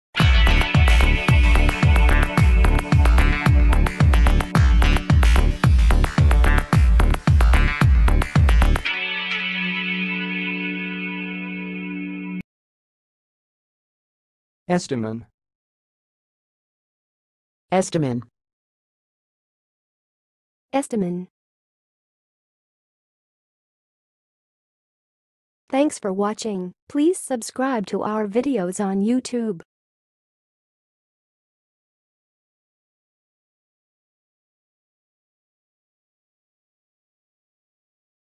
How to say "estamin"! (High Quality Voices)
Watch in this video how to say and pronounce "estamin"!